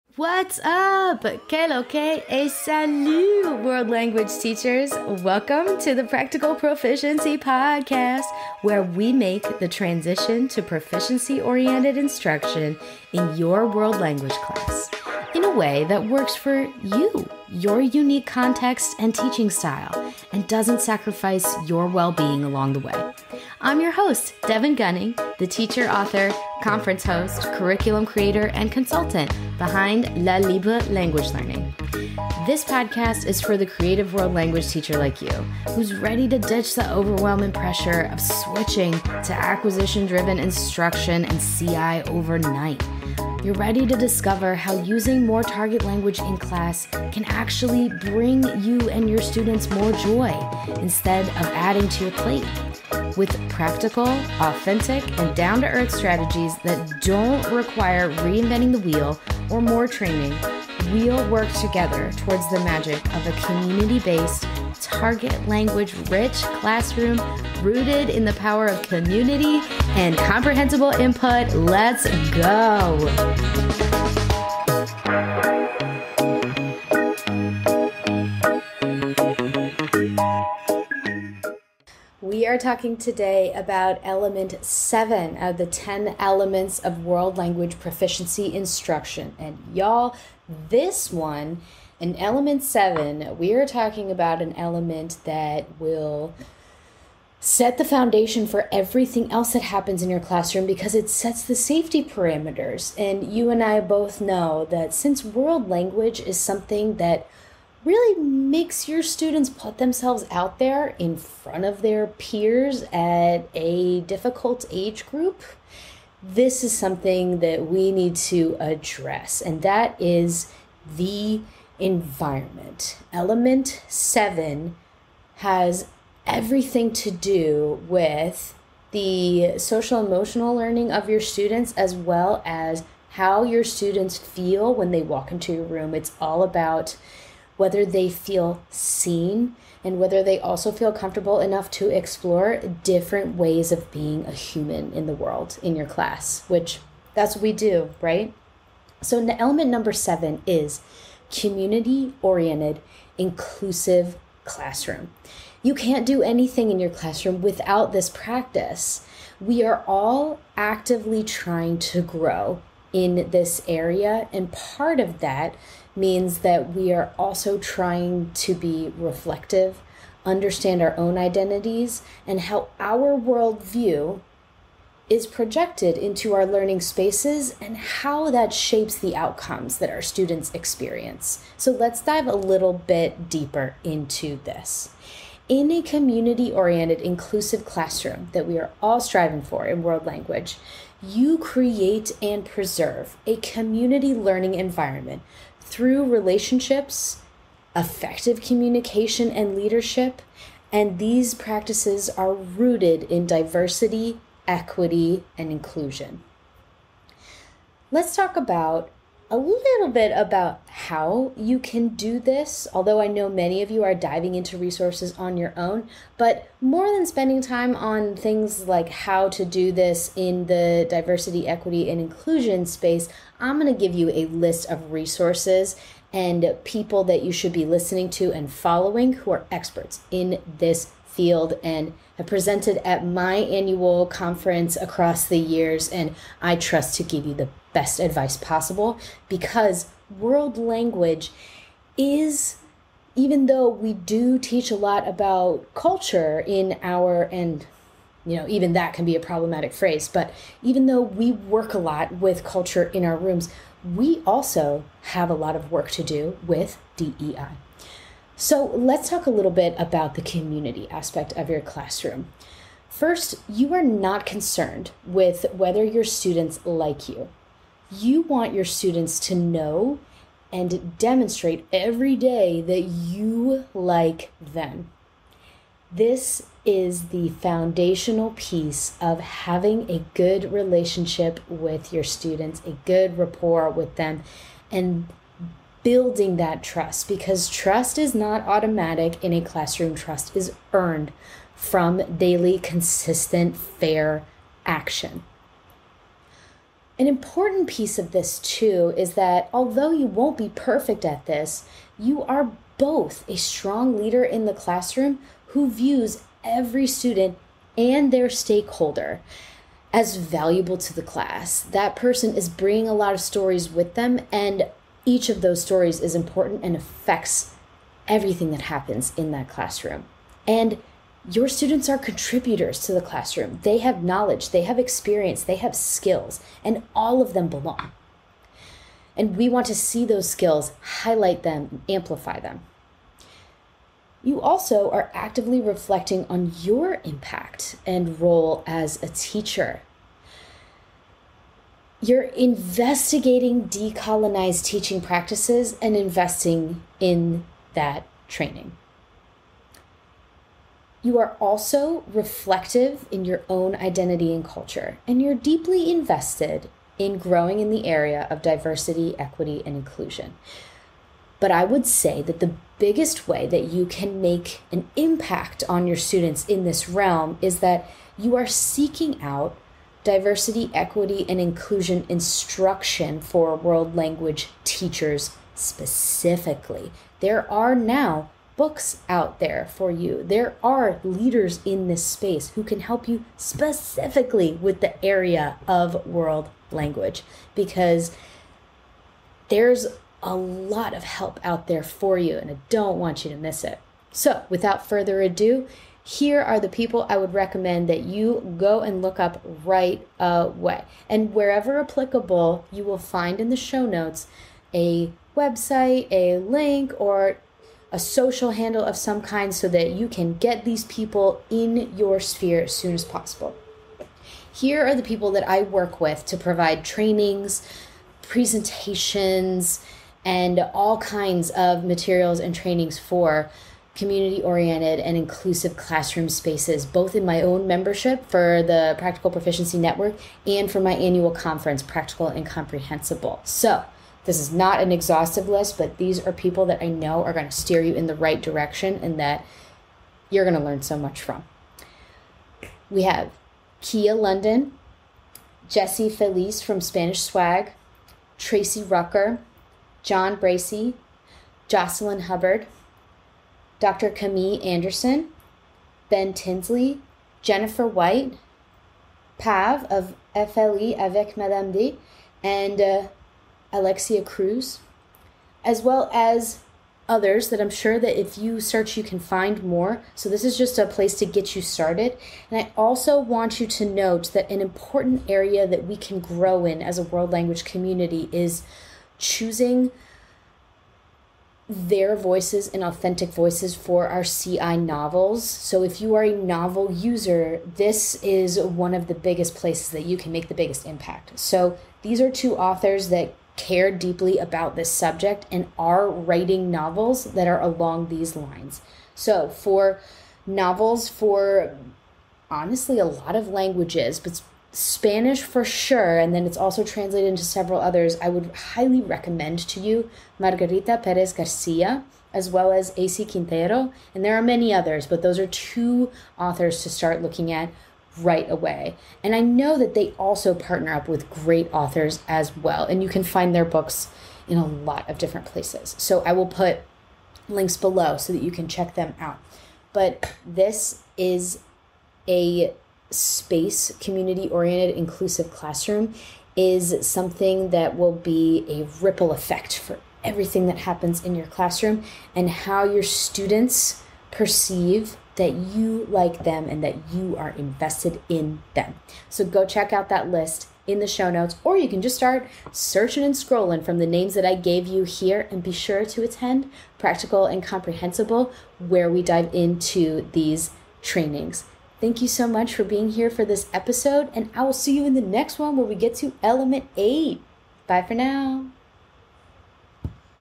#10 - Element 7: Inclusive and Community Oriented Spaces | 10 Elements of Proficiency-Oriented Wo...
In this episode, we discuss a list of the people I enjoy learning from to further my journey into equity-driven education. These are the trusted speakers, writers, and teachers to follow and keep in your feed!

Head here to the accompanying blog post for the list of DEI speakers and leaders in the world language space.

Go to Blog Post of the 10 Elements of Proficiency Oriented World Language Instruction (contains image printout)